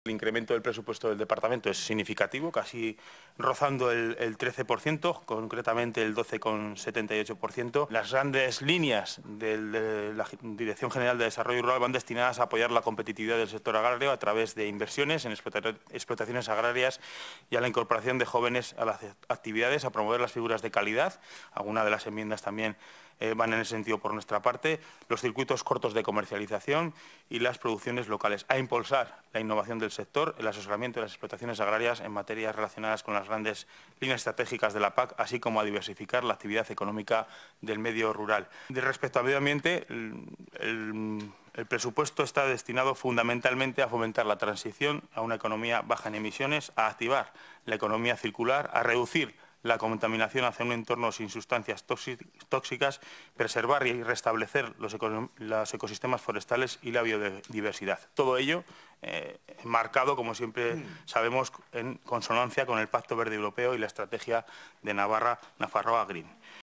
Pablo Azcona valora el incremento del 13% en el presupuesto de Desarrollo Rural y Medio Ambiente
El parlamentario de GeroaBai, Pablo Azcona, valora que los PresupuestosNavarra2023 en materia de DesarrolloRural y MedioAmbiente se incrementan en un 13%, con el apoyo a la competitividad del sector agrario y el fomento de la transición a una economía baja en emisiones como líneas maestras.